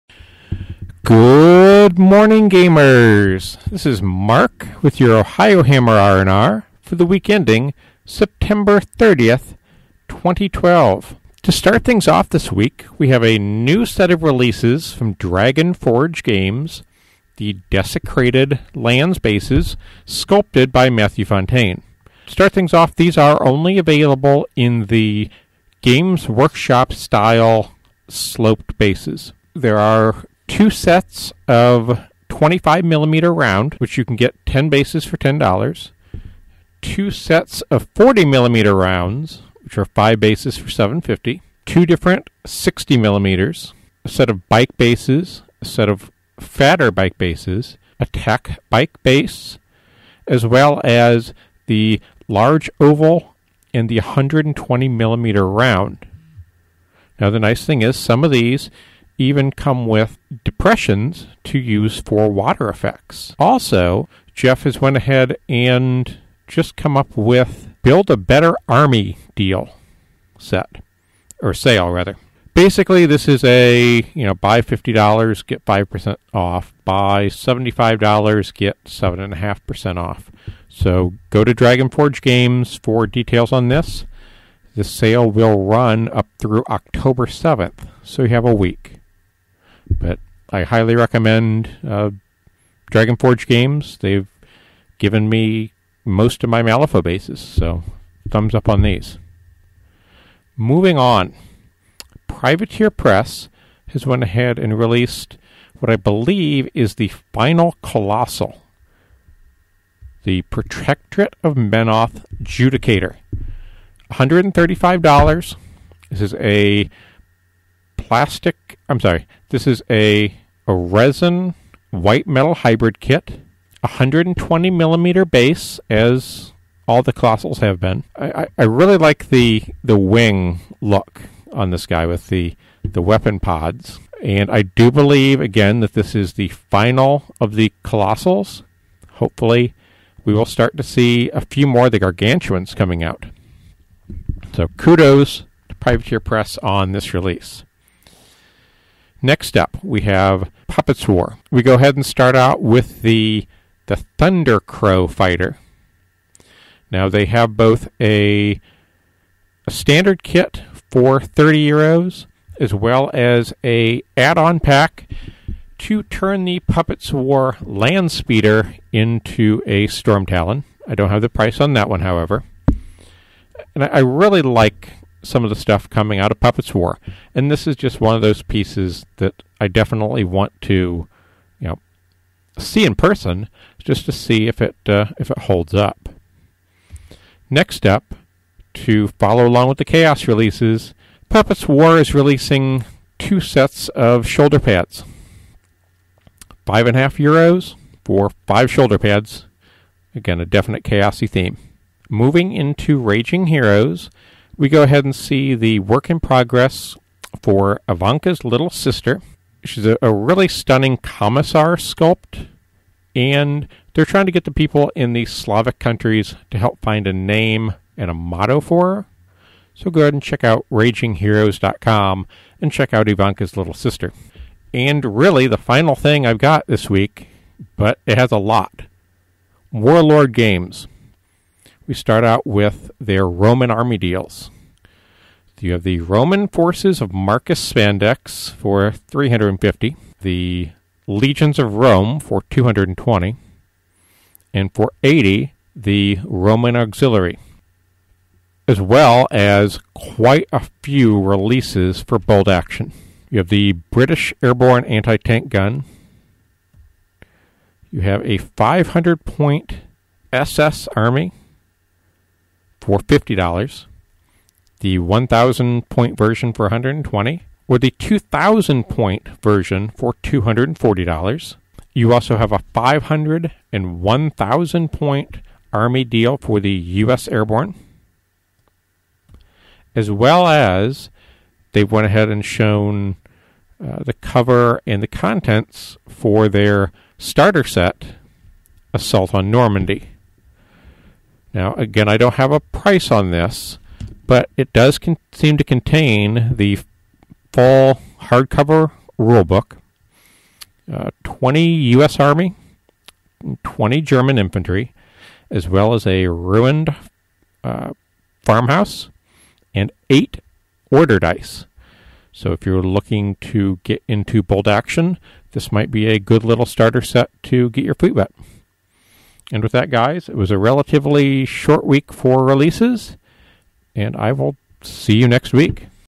Ohiohammer RNR - September 30th, 2012
This week we cover Puppets War, Privateer Press and Warlord Games